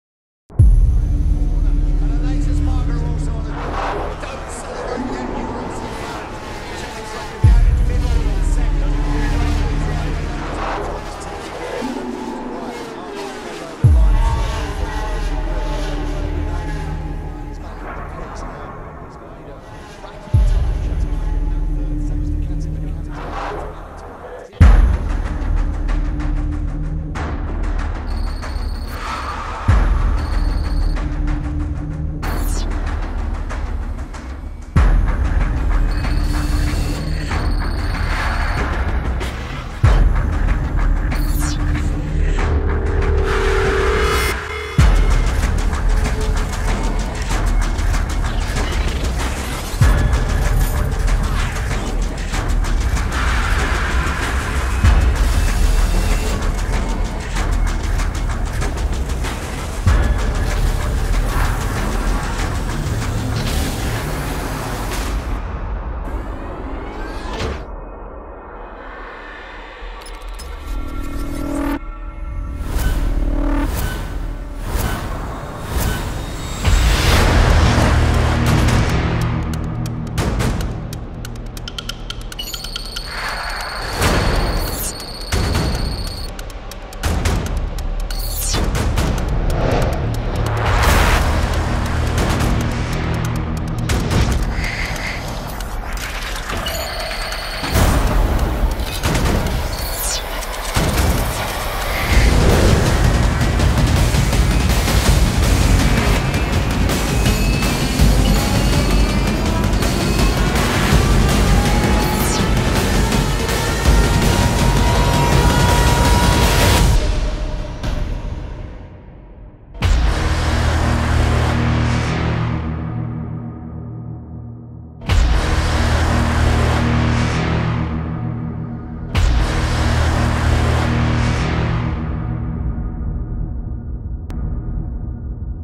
Bimota EB1 Concept - CGI animation
Check out our last CGI animation for the BIMOTA EB1 concept.
Realized by four digital designer students at ISD Valenciennes, this movie is the result of a ten months work.